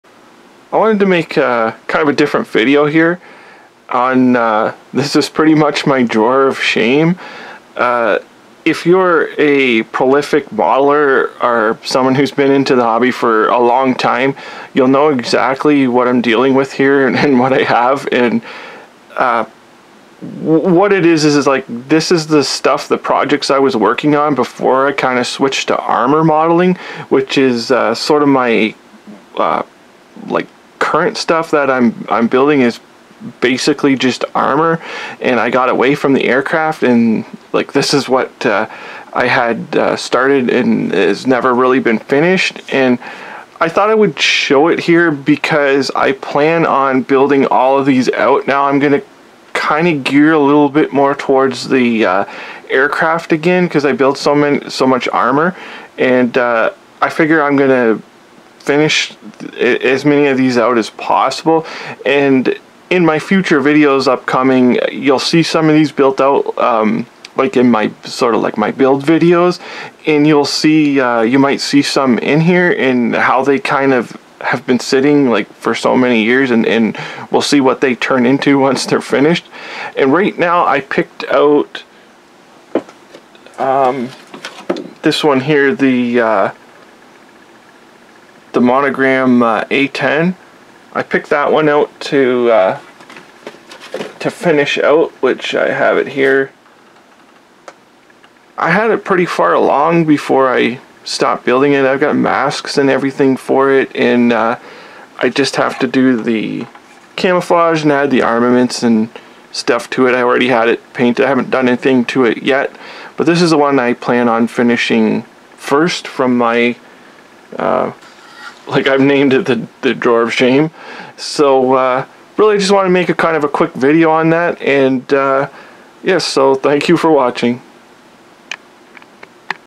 Unfinished model kit projects
Unfinished model kit aircraft/projects i plan on getting back to and finishing.